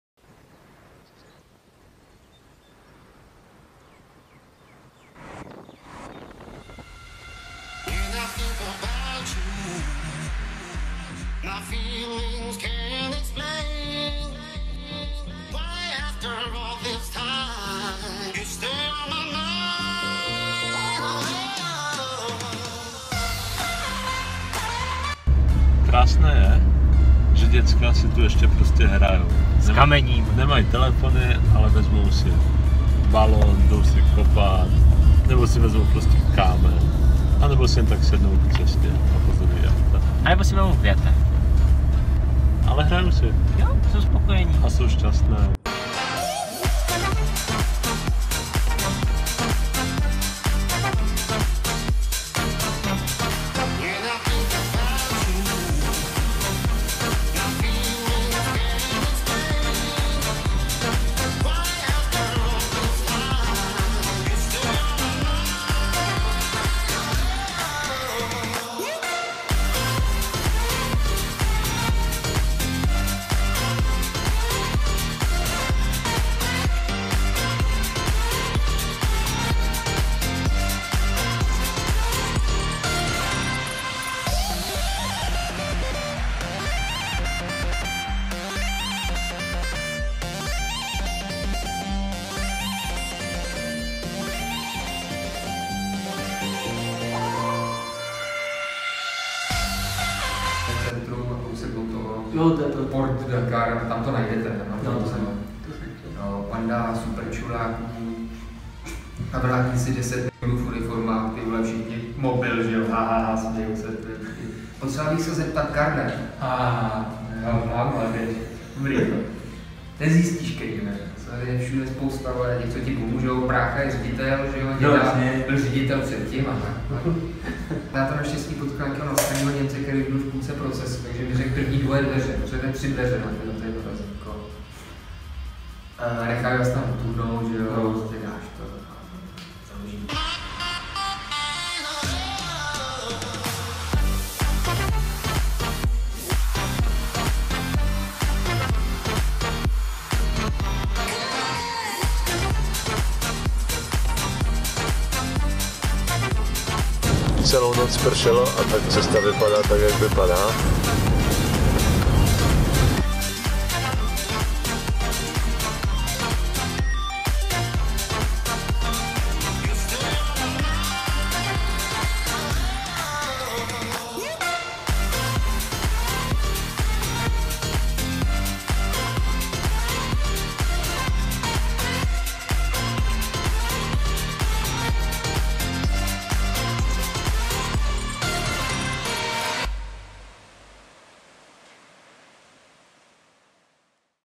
Díl 3. Maroko a povodně
Cesta z Tangeru do hor za Marakéšem, kde nás v noci překvapil déšť a ráno povodně.